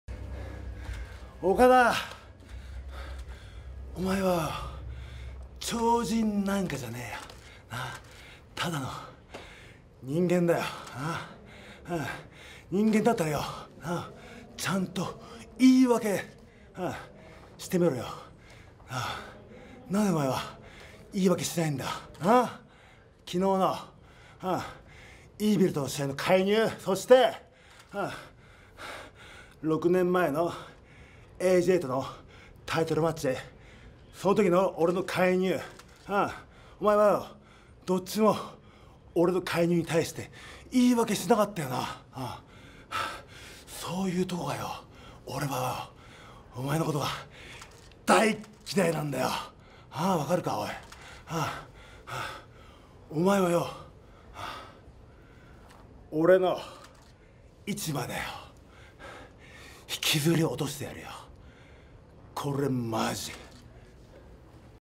Yujiro Takahashi wants a match with Okada years in the making!
Match 4 : Goto & Okada vs. Ishimori & Yujiro